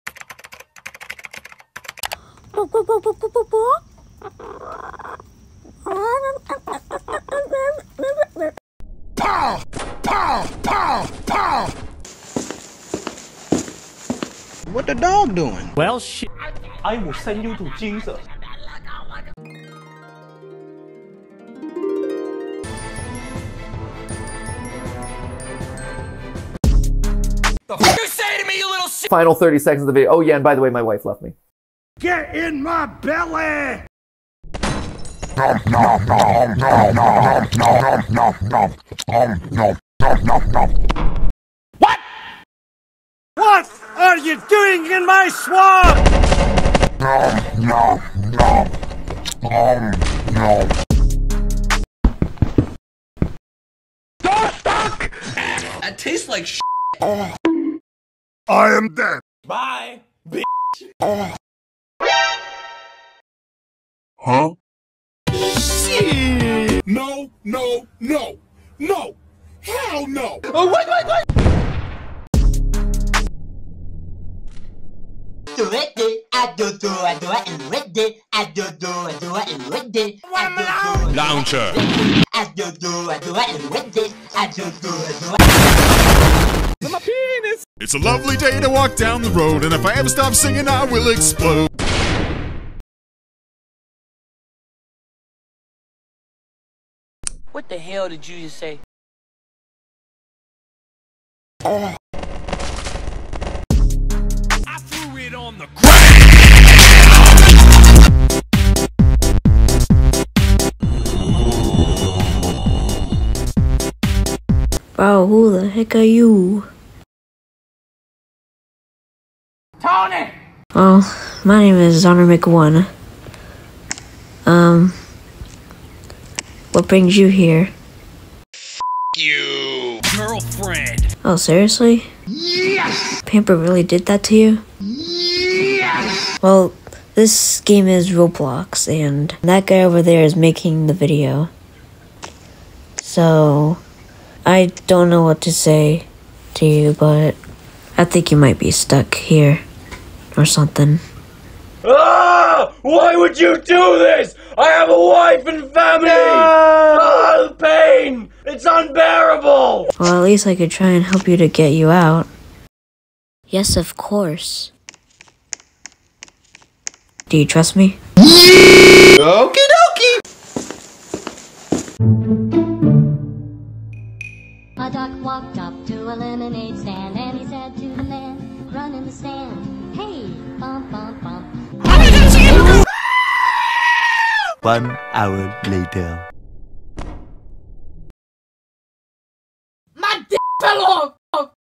If Corny was in… Michael Bicycle Videos (Pre-Episode)
Corny is watching videos on Pampers computer, minding his own business until Pamper comes in the room and sends him to my YT channel :D